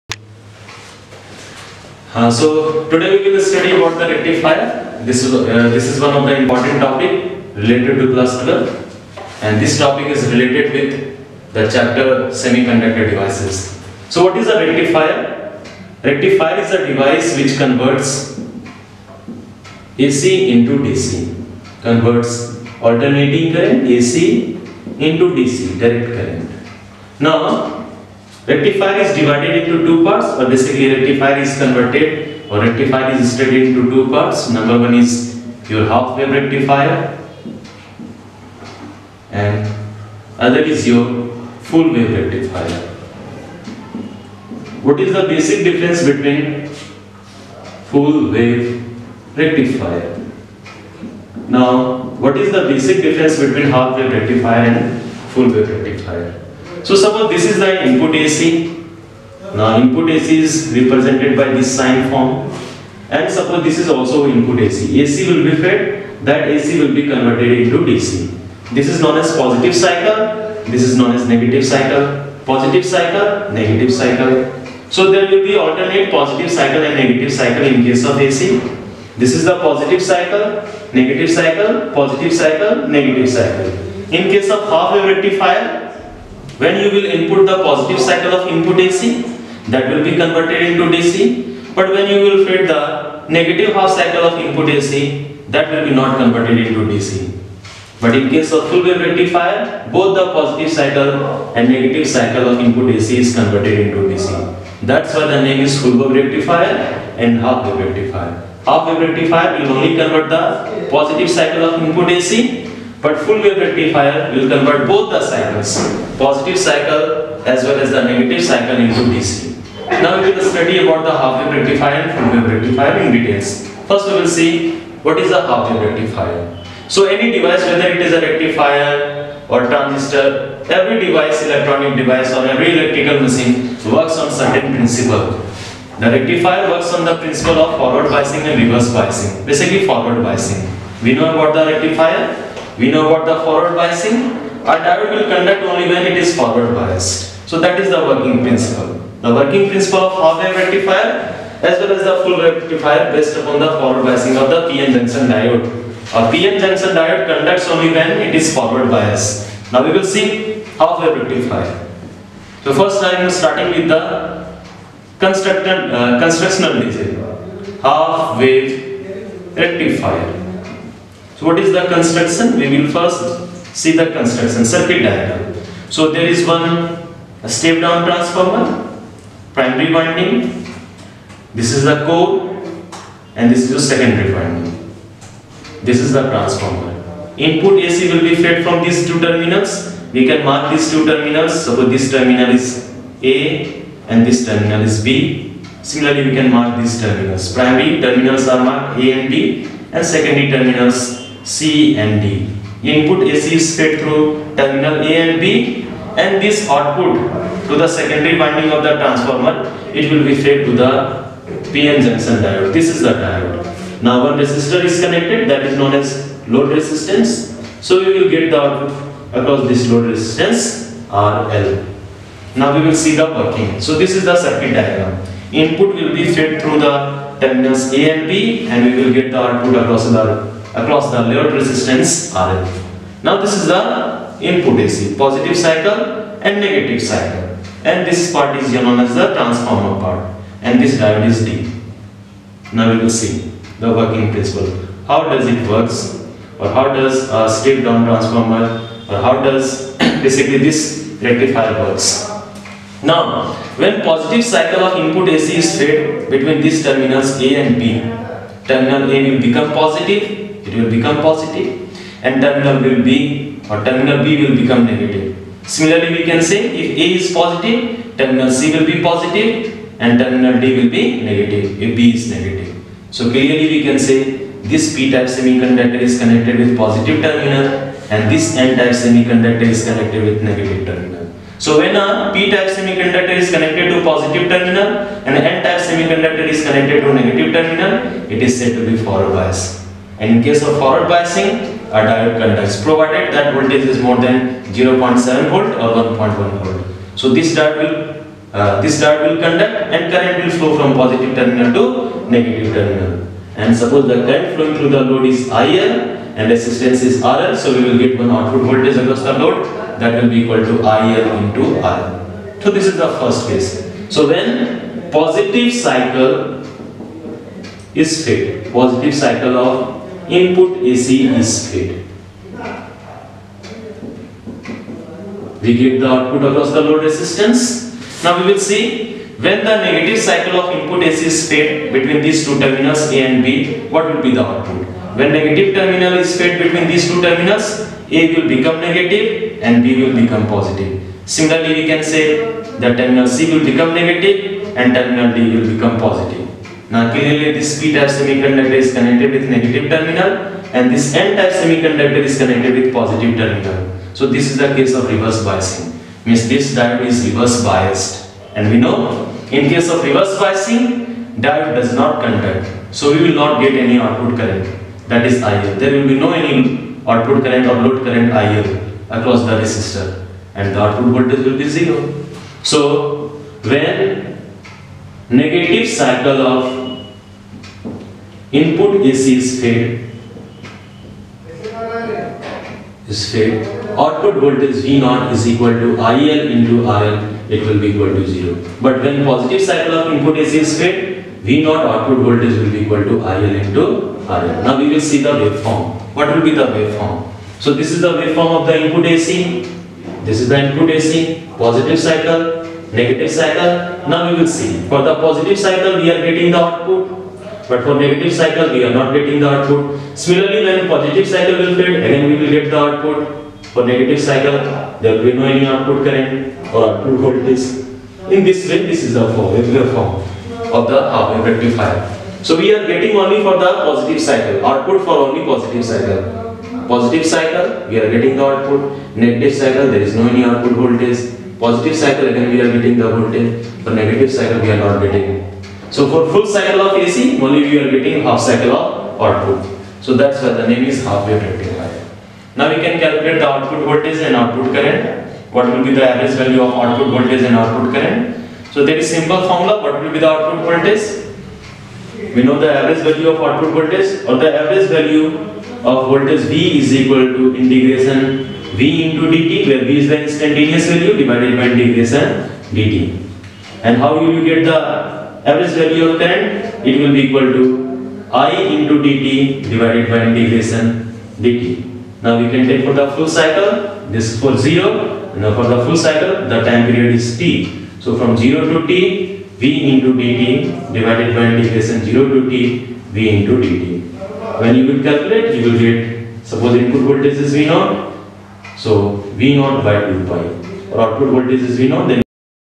Half wave rectifier | Class 12 Physics | NEET-JEE
In this video you will learn the basic principle of working of a rectifier